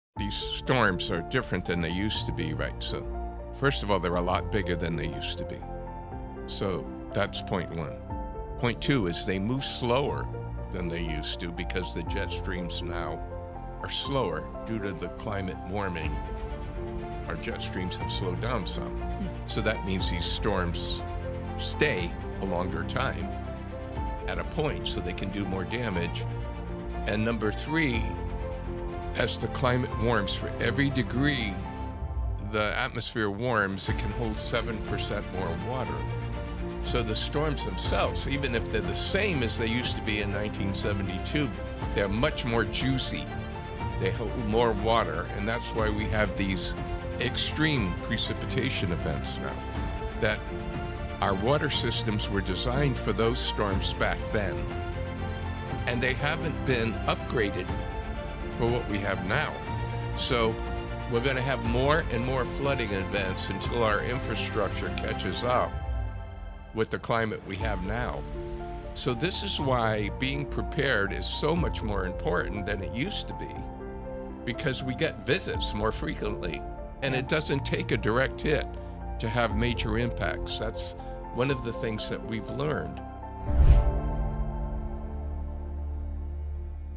Why Storms Are More Dangerous Now | Bob Bunting
⚠️ Storms have changed — and not in a good way.
They're bigger, slower, and carry more water than ever before. 🌧️ Why? Climate change. And it’s overwhelming our infrastructure.

🌊 Floods are striking farther and wider. Even without a direct hit, entire communities are being devastated. We dive into how water systems are struggling to keep up — and what we need to do now to prepare for what’s coming.

Are you and your community ready?

🎧 Tap in for expert insights on storm preparedness, climate resilience, and the future of disaster planning.
📅 New episodes drop every Thursday.